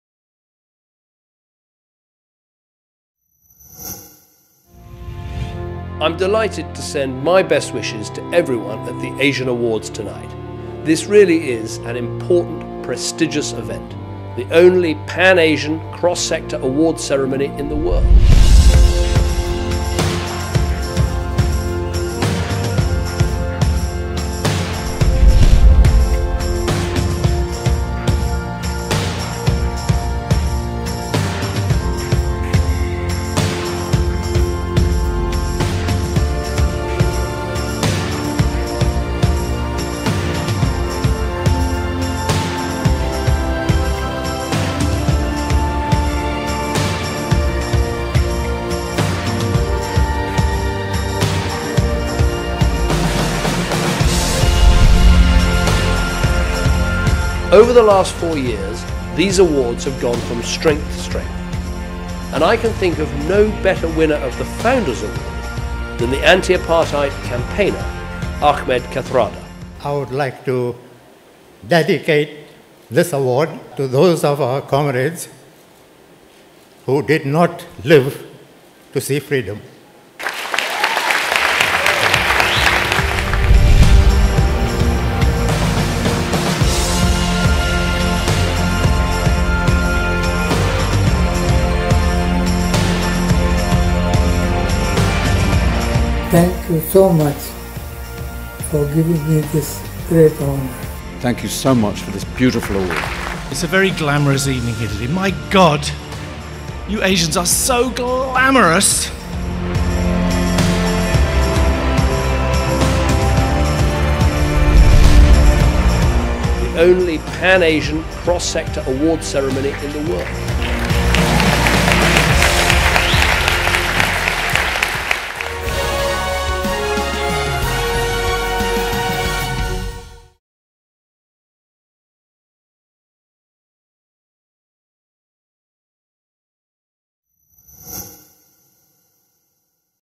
The 5th Asian Awards Pre-event Promo
The 5th Asian Awards will take place on 17th April 2015 at London's Grosvenor House Hotel. This video looks back on some of the great winners that the awards have honoured.